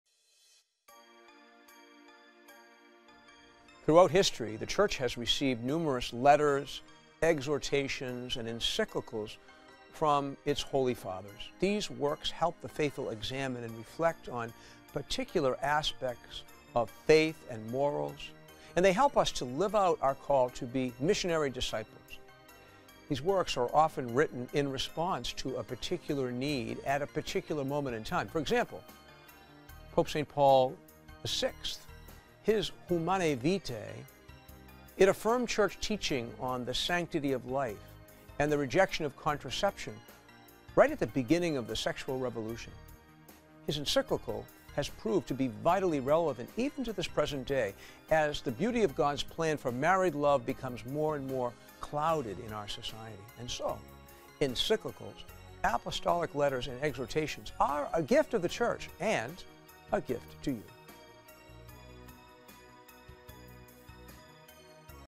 18 | Exhortations & Encyclicals | 25 Gifts of the Church
Throughout history, the Church has received numerous letters, exhortations, and encyclicals from its holy fathers. These works help the faithful examine and reflect on particular aspects of faith and morals, and aid us in living out our call to be missionary disciples. These works are often written in response to a specific need at a particular moment in time. For example, Pope Paul VI’s Humanae Vitae affirmed Church teaching on the sanctity of life and the rejection of contraception right as the sexual revolution began. His encyclical has proven to be vitally relevant up to the present day, as the beauty of God’s plan for married love becomes more and more clouded in society. Encyclicals/apostolic letters and exhortations are a gift of the Church and a gift to you.